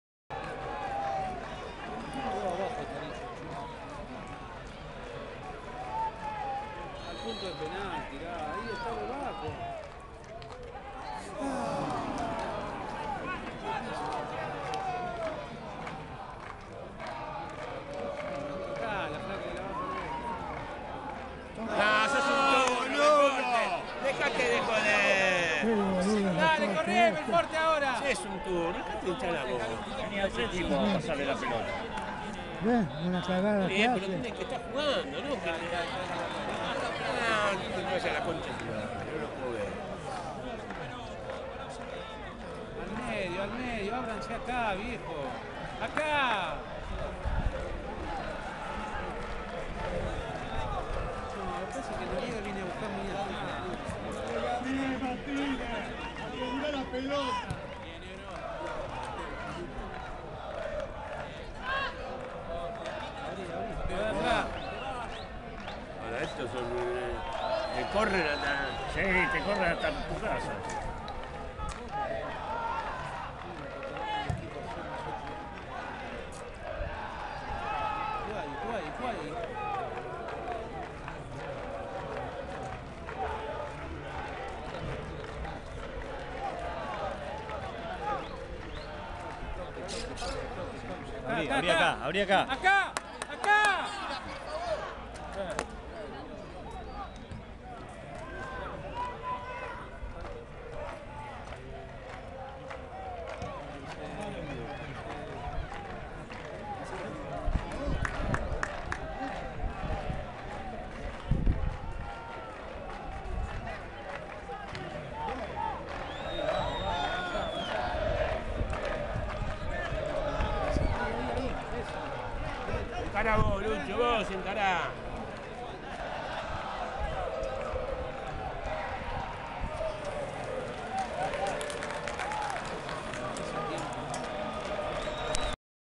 2014-04-13 Los Andes Vs Fenix. (03)
LA PELOTA IBA Y VENIA Y NO PASABA NADA EN EL PARTIDO, INTENTABAN LOS DOS EQUIPOS PERO NADA DE NADA.